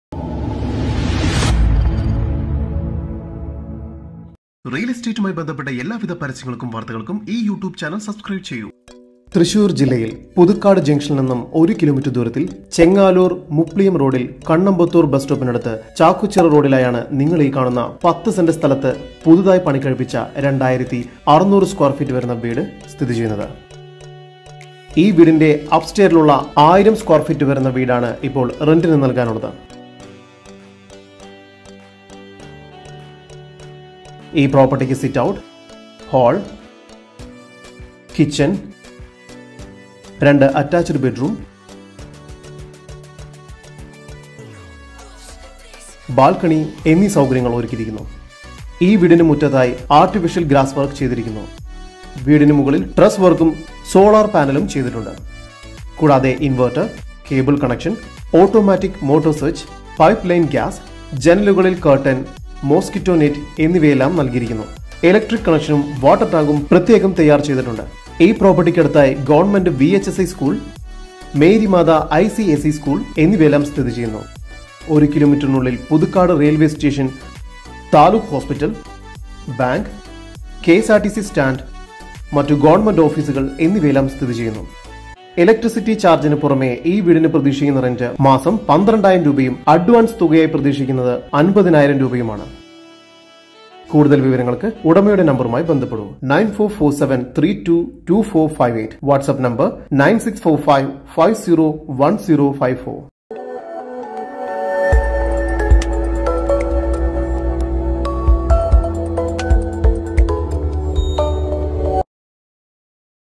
House for Rent in Thrissur | 12000 | Contact Owner
House for Rent In Thrissur
Puthukkadu
Chengaloor
2600 sqft
3 Bhk
Near VHSE School
Near Mary Matha ICSE School
1 km from Puthukkadu Railway Station
1 km from Taluk Hospital
1 km from Bank
1 km from KSRTC Bus Stand
Monthly Rent 12000/-
Advance 50000
For More Details Call Or WhatsApp
Owner Name   :Mr.  Anto
Owner Number :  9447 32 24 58
             :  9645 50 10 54 (Whatsapp)

വീഡിയോ ലൈക് ചെയുക
സുഹൃത്തുക്കൾക്ക് മാക്സിമം ഷെയർ ചെയ്യുക

Published by

JUS MEDIAS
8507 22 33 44